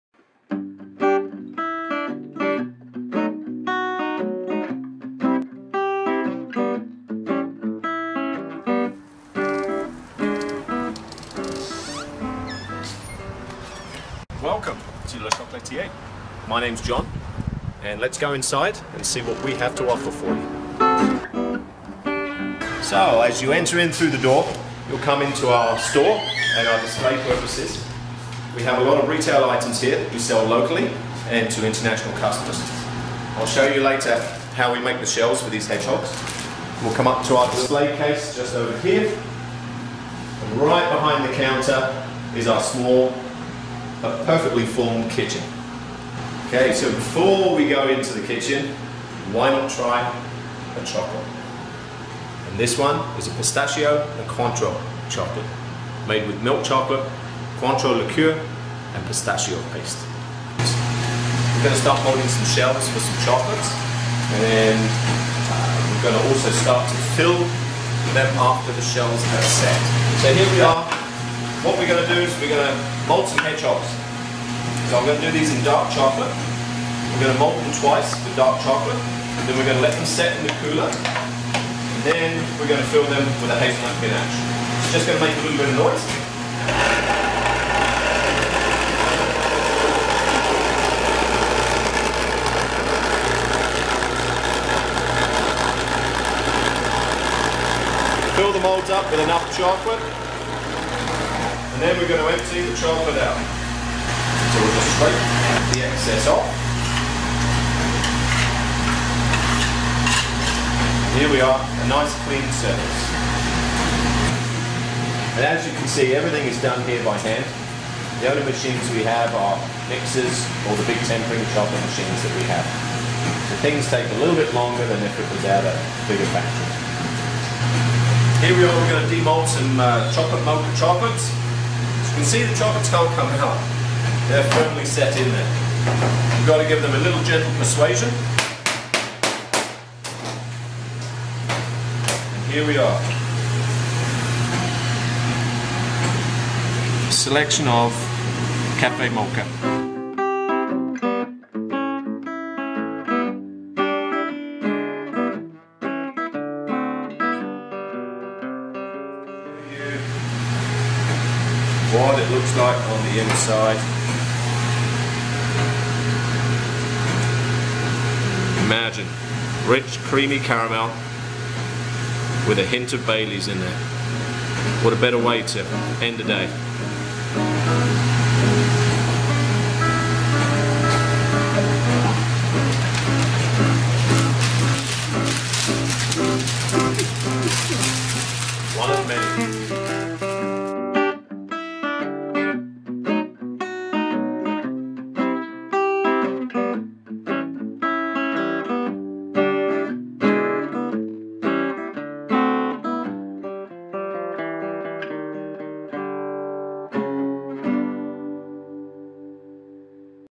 Le Chocolatier • Welcome
We create our chocolate confections at our independent chocolate shop in Canmore, Alberta (close to Banff) in the Canadian Rockies. Our 1,800 sq. ft. shop has a fully-stocked chocolate retail storefront plus a glass wall for visitors to watch the magic happen.